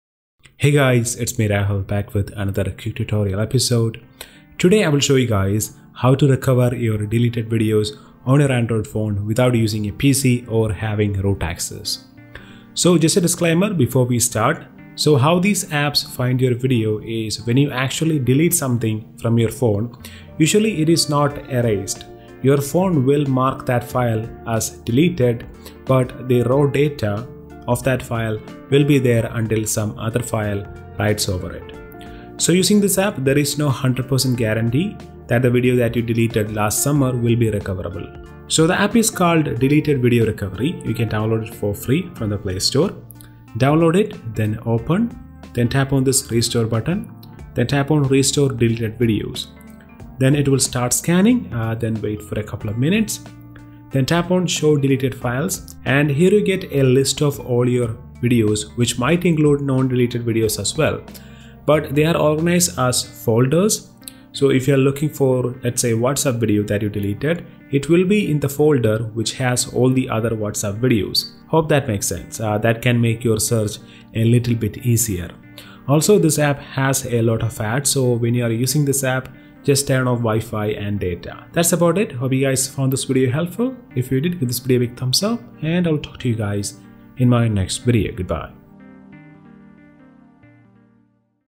How to Restore, Recover Deleted Videos On Android Phone Without Root and Without PC : Quick Tutorial
In this video I will show you How to Restore, Recover Deleted Videos On Android Phone Without Root and Without PC using a small and simple application